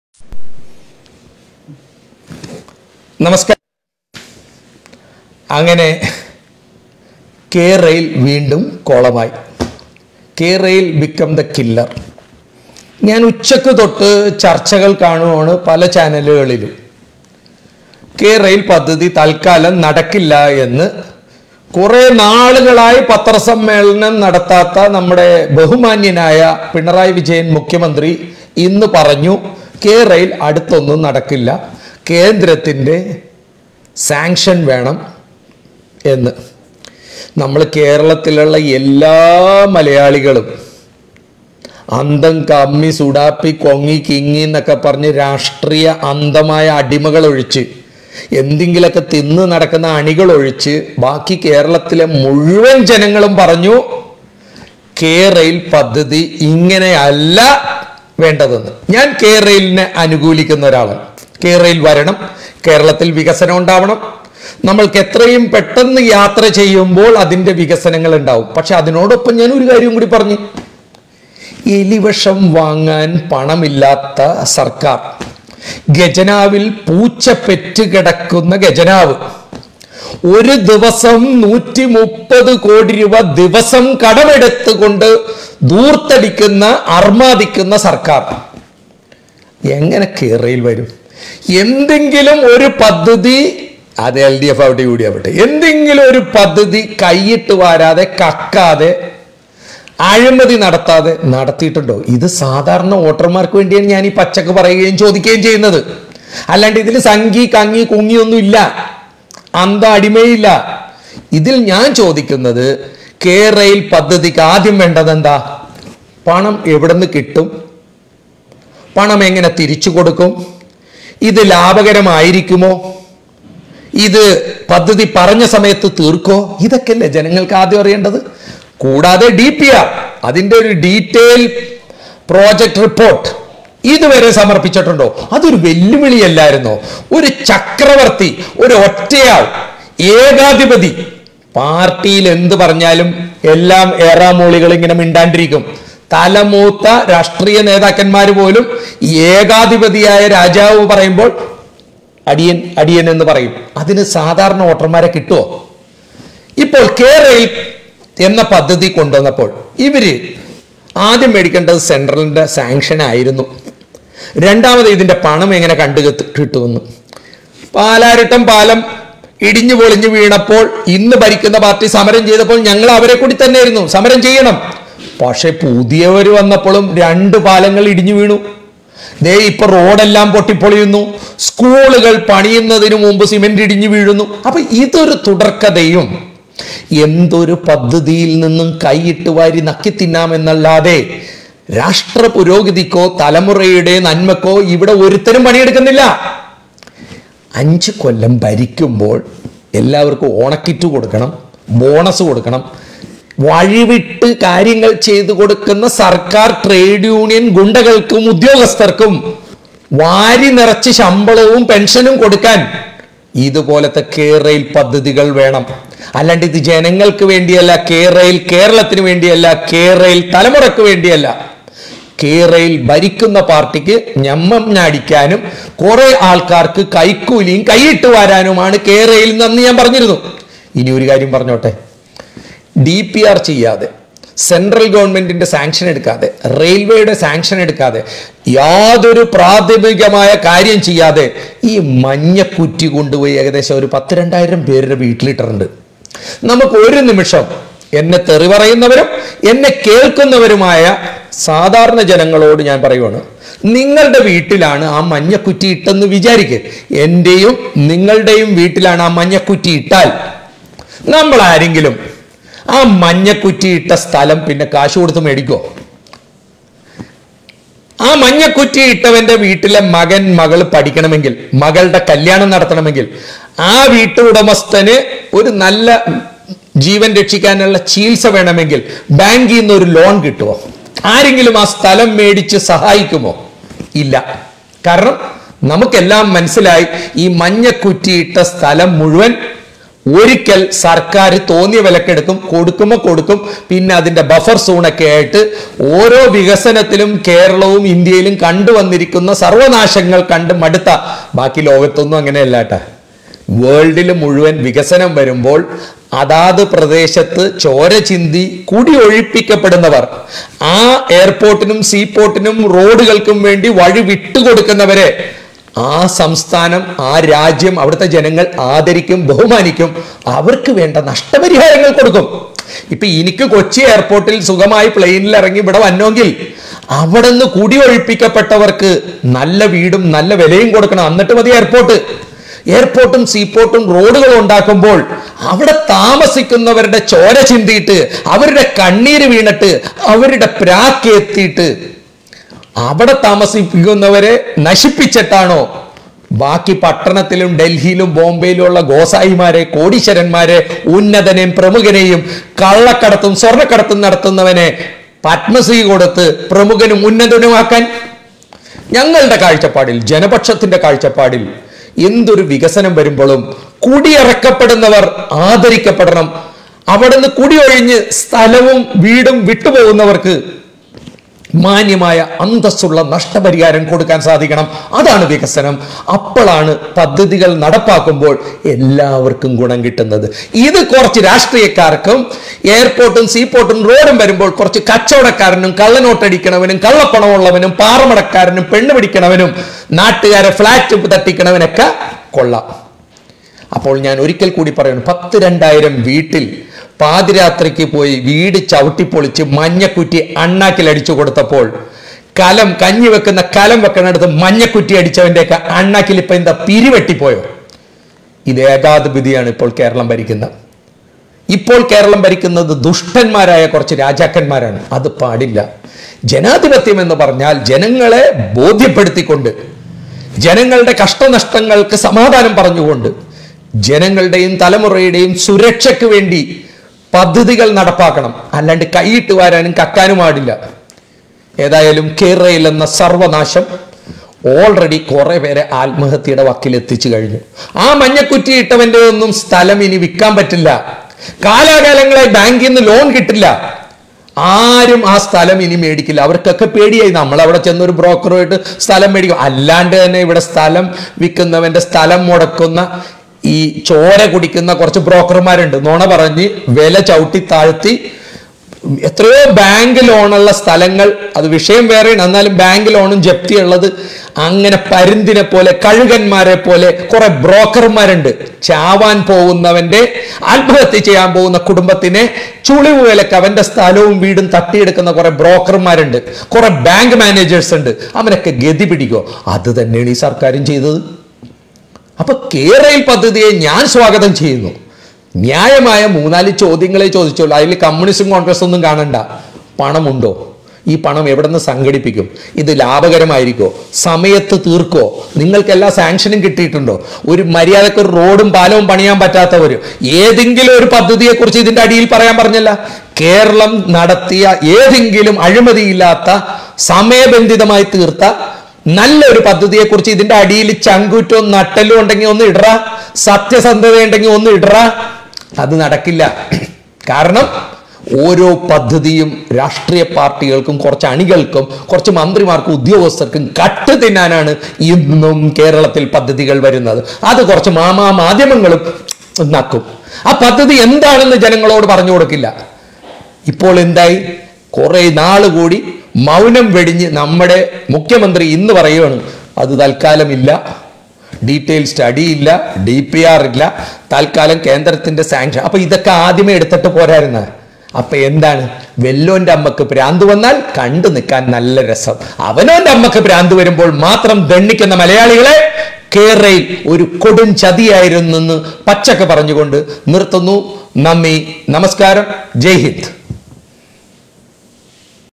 K- RAIL THE KILLER പാവങ്ങൾ പെട്ടു
എത്ര കുടുംബം വഴിയാധാരമാക്കി
നശിപ്പിച്ചു 😥 K Rail കക്കാൻ